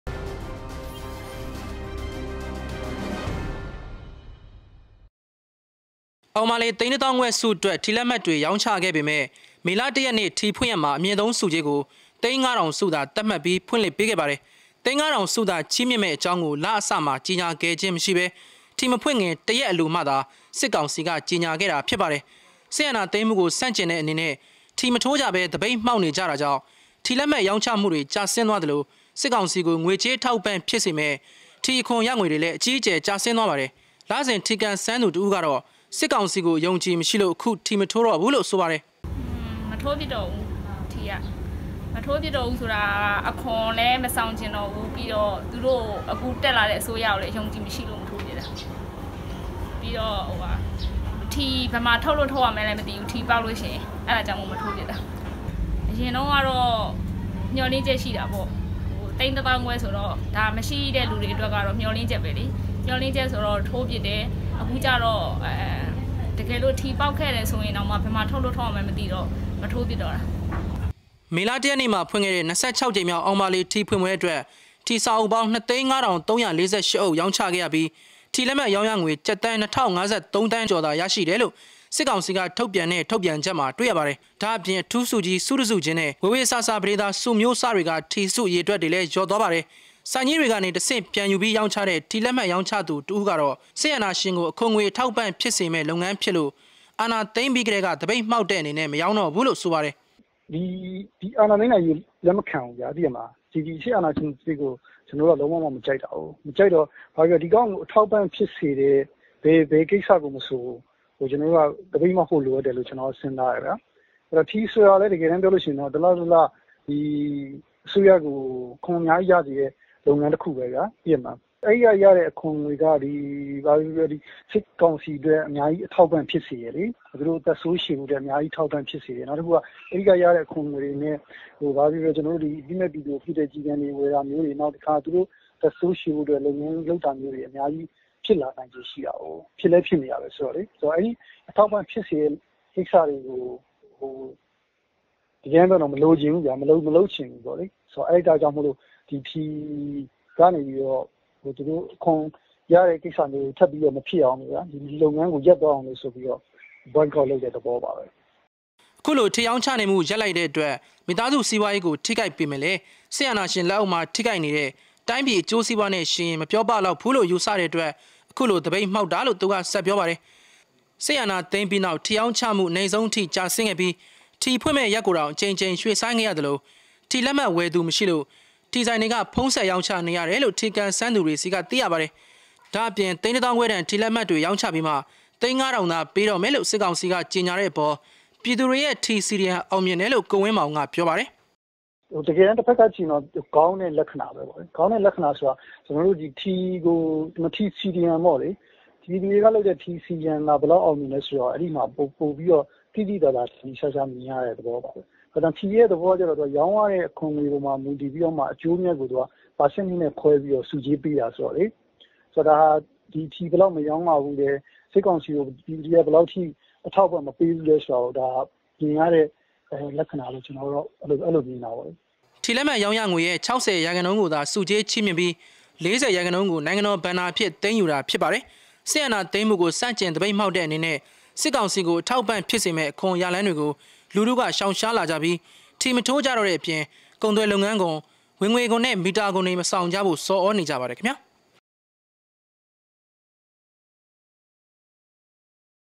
ပြည်သူတွေရဲ့ ထီ CDM ‌စစ်ကောင်စီကို ထိခိုက်မှုရှိ
စစ်အာဏာသိမ်းမှုကို ဆန့်ကျင်သည့်အနေဖြင့် ထီမထိုးကြဘဲ သပိတ်မှောက် နေကြသောကြောင့် ထီလက်မှတ်ရောင်းချမှုများကျဆင်းသွားသလို စစ်ကောင်စီကိုငွေကြေး အထောက်အပံ့ဖြစ်စေမည့် ထီအခွန်ရငွေများလည်း အကြီးအကျယ်ကျဆင်းသွားပါသည်။

ထီလက်မှတ်ရောင်းရငွေ၏ ၆၀ ရာခိုင်နှုန်းကိုသာ ဆုကြေးချီးမြှင့်ပြီး ၄၀ ရာခိုင်နှုန်းကို နိုင်ငံတော်အခွန်အငွေဖြစ်သတ်မှတ်ထားသည်။ စစ်အာဏာသိမ်းမှုကို ဆန့်ကျင်သပိတ်မှောာက်သည့်အနေဖြင့်စစ်ကောင်စီကို အထောက်အပံ့ဖြစ်မည့် အခွန်ရလမ်းများကို လူထုက ရှောင်ရှားလာကြပြီး ထီမထိုးကြတော့သည့်အပြင် ကုန်သွယ်လုပ်ငန်းခွန်၊ ဝင်ငွေခွန်နဲ့ မီတာခွန်များမပေးဆောင်ကြရန် ဆော်ဩနေကြပါသည်။